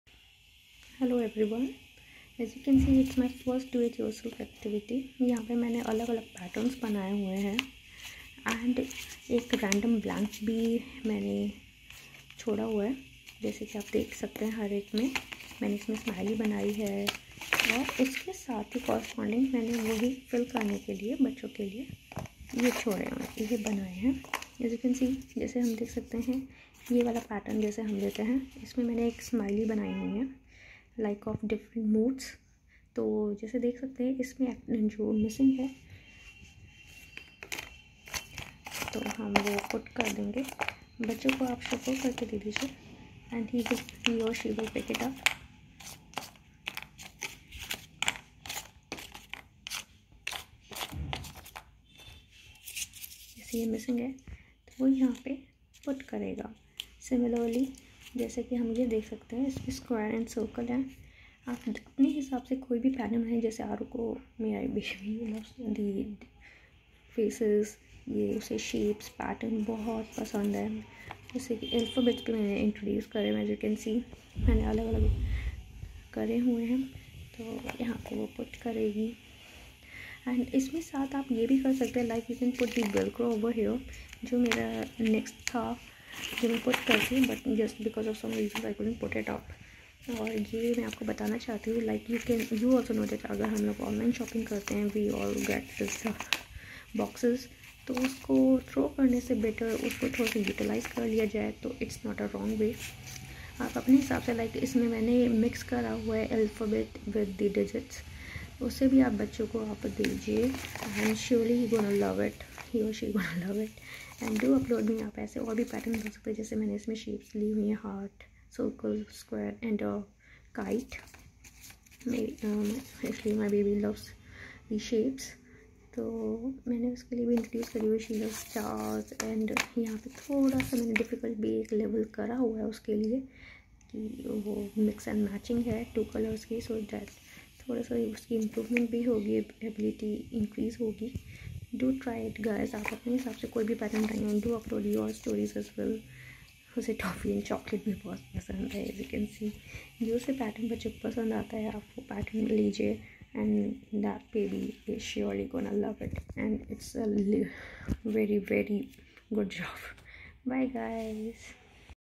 1st DIY (pattern matching)
Its a pattern matching with a random blank space. Pattern can be made by your own choice n let the fun begin with your kid. Its a must activity for 2.5yr old kid. Its fun engaging game along with envirommental friendly.
It enhances motor skills of baby. It sharpens shape, colour & paytern recognition skills too.
Do try it with your imagination